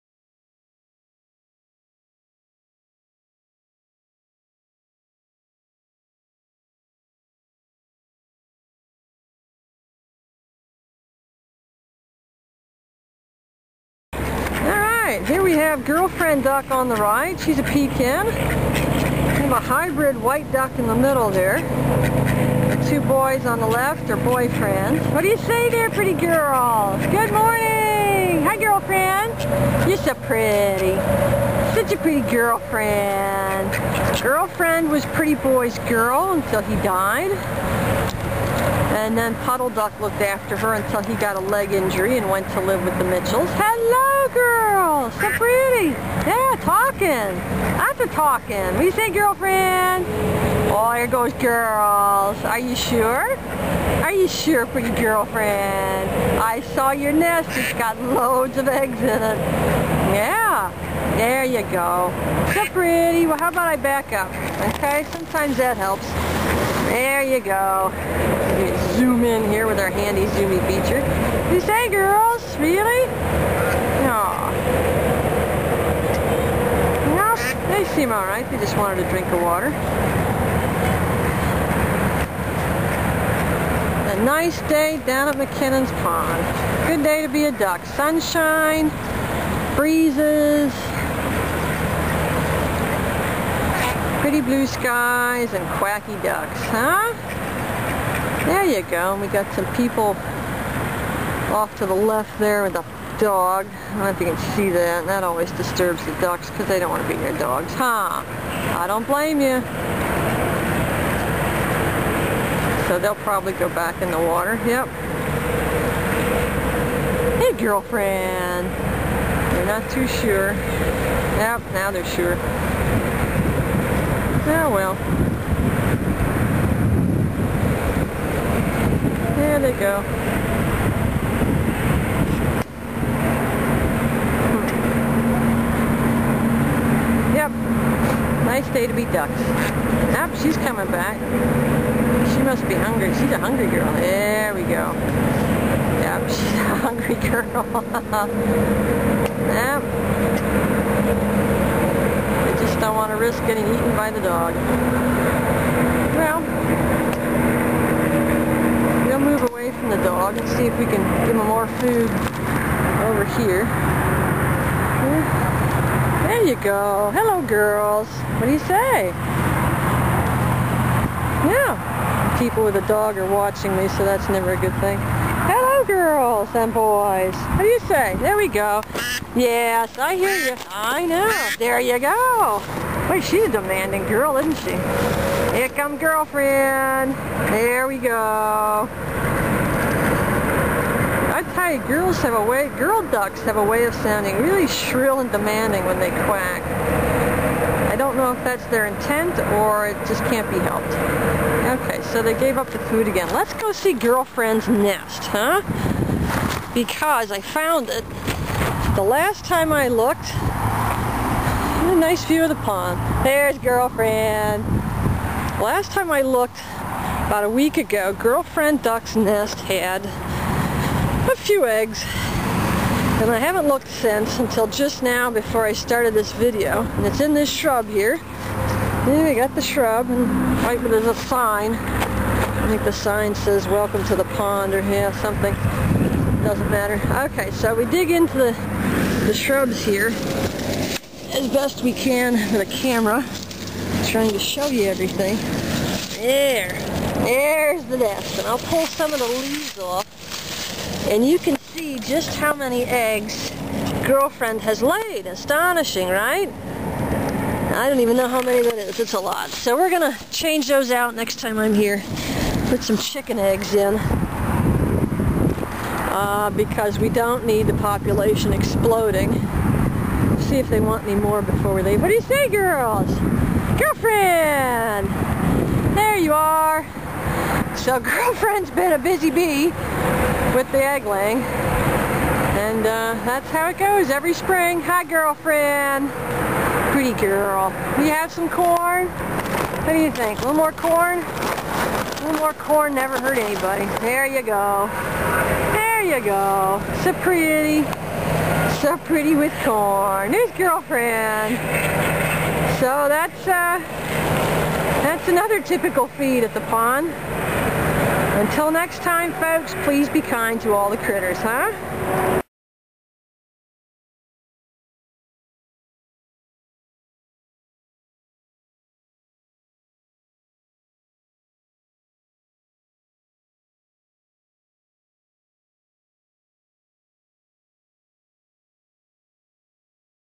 Meet Girlfriend Duck!.wmv
Meet white Pekin "Girlfriend Duck" and listen as the ducks quack for the Crazy Critter Lady! Then check out the contents of Girlfriend Duck's nest - she's been one busy duck!